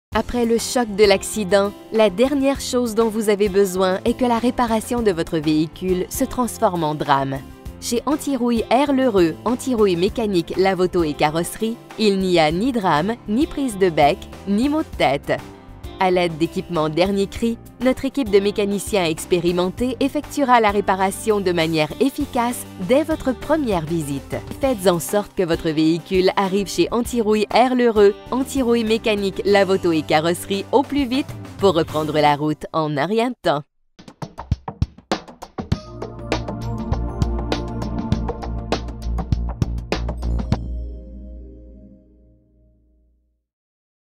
Antirouille R. l'Heureux, Antirouille, Mécanique, Lave-Auto & Carrosserie
"Après le choc de l'accident, la dernière chose dont vous avez besoin est que la réparation de votre véhicule se transforme en drame. Chez Antirouille R. l'Heureux, Antirouille, Mécanique, Lave-Auto & Carrosserie, il n'y a ni drames, ni prises de becs, ni maux de tête. À l'aide d'équipements dernier cri, notre équipe de mécaniciens expérimentés effectuera la réparation de manière efficace dès votre première visite. Remorquez-le, poussez-le, conduisez-le si vous le pouvez, mais faites en sorte que votre véhicule arrive chez Antirouille R. l'Heureux, Antirouille, Mécanique, Lave-Auto & Carrosserie au plus vite, pour reprendre la route en un rien de temps!"